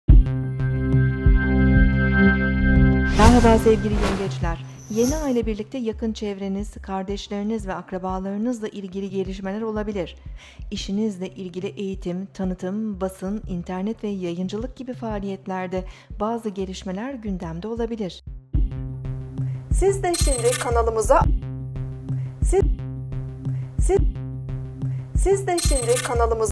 YENGEÇ günlük yorumu 1 Eylül 2016 Perşembe
Yengeç Burçları Merhaba Bugün 1 Eylül 2016 Perşembe
Yeniay ile birlikte yakın çevreniz, kardeşleriniz ve...